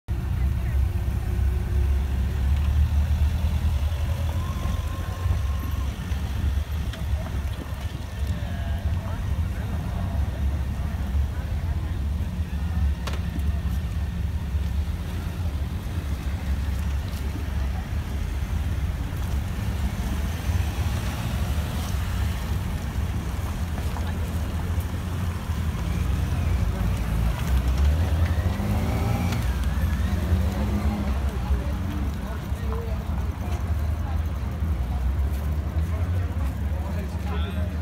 Kronprins kortege og politi forlader Flakhaven
Kronprins Japan. Foto: Ivar Juel nordentoft